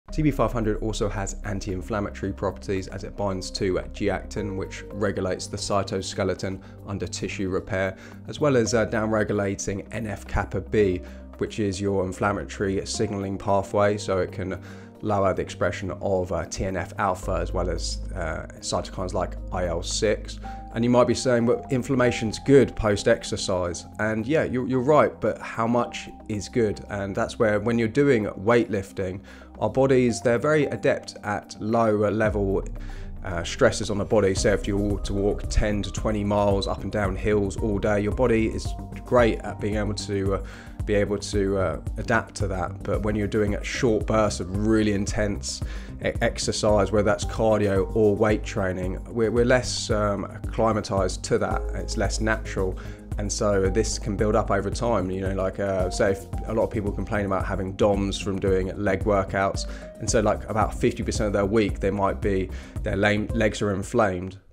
What Happens If You Use TB-500 Muscle Recovery?
In this video, I'm discussing the benefits of TB500 for promoting tissue repair and reducing scar formation. TB 500 is one of the best peptides for healing due to its anti-inflammatory properties and ability to improve wound healing. It is often stacked with BPC 157, so let's dive in! In this video, we're diving into the world of peptides and exploring the potential benefits of TB-500 for muscle gain and recovery. As a fitness enthusiast, you're likely no stranger to the importance of muscle repair and growth, and TB-500 has been gaining popularity as a peptide stack that can help you heal faster and build muscle more efficiently. But what exactly is TB-500, and how does it work? We'll take a closer look at the science behind this peptide, its benefits, and optimal dosing to get the most out of your workout routine. Whether you're a seasoned athlete or just starting out on your fitness journey, this video will give you a comprehensive understanding of how TB-500 can help you achieve your goals. From biohacking recovery to peptide stacks, we'll cover it all. So, let's get started and explore the world of TB-500!